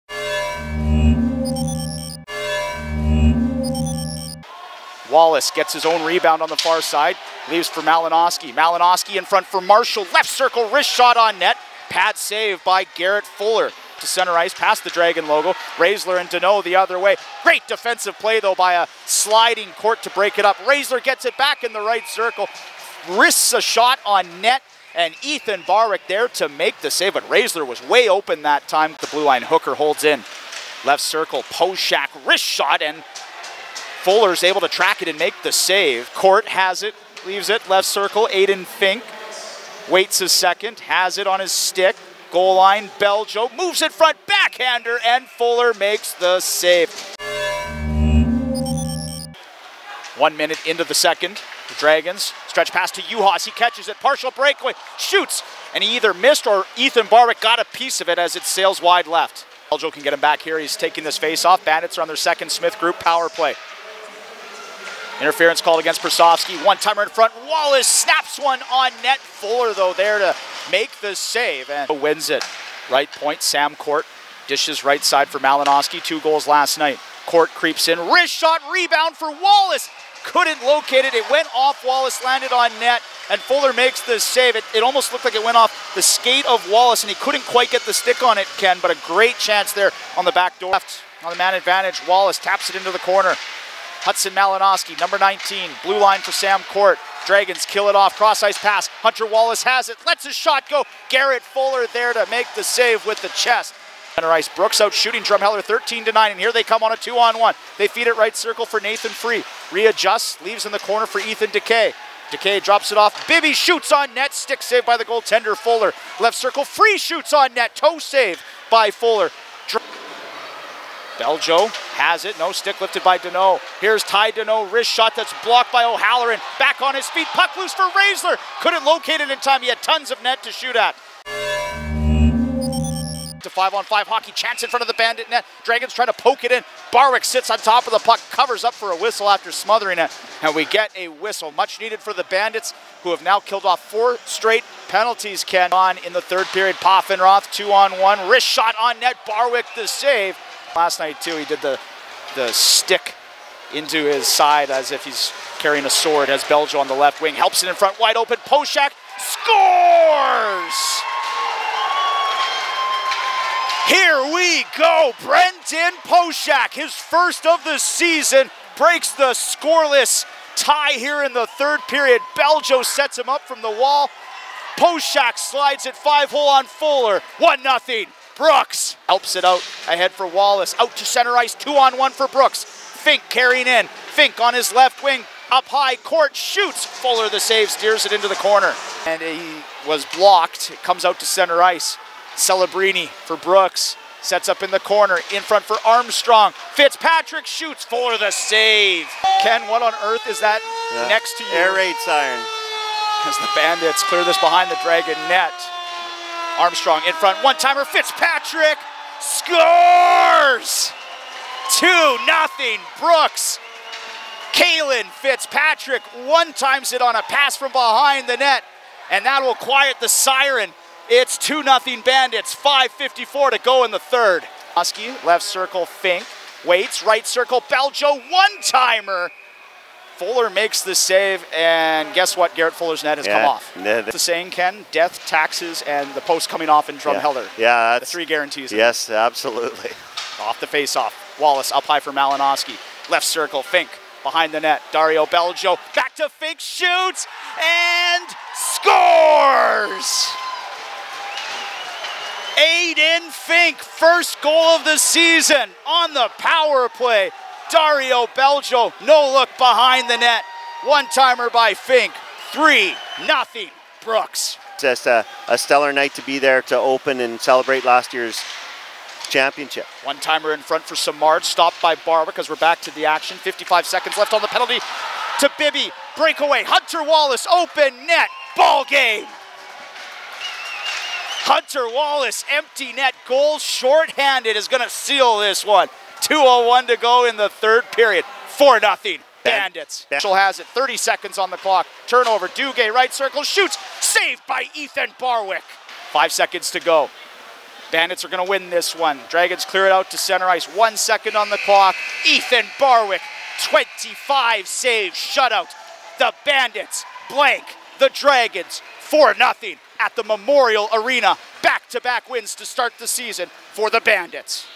Highlights: Brooks 4 @ Drumheller 0 (September 17, 2022)
Brendan Poshak broke a scoreless 3rd period, Ethan Barwick made 25 saves, and the Brooks Bandits prevailed in a 4-0 blanking of the Drumheller Dragons on Saturday at the Memorial Arena.